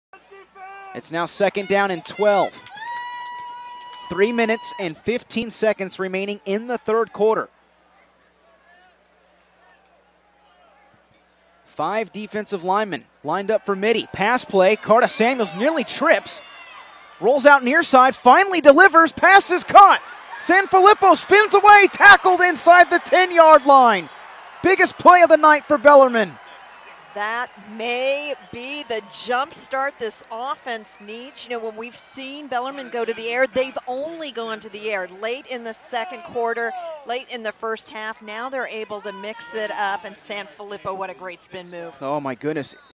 #10 KJ Carta-Samuels connects with #25 Joey Sanfilippo for a big 25 yard reception
Escaping defenders KJ Carta-Samuels finds Joey Sanfilippo for a big 25 yard reception and 1st down for the Bellarmine Bells against the Archbishop Mitty Monarchs in this CIF Central Coast Section match up.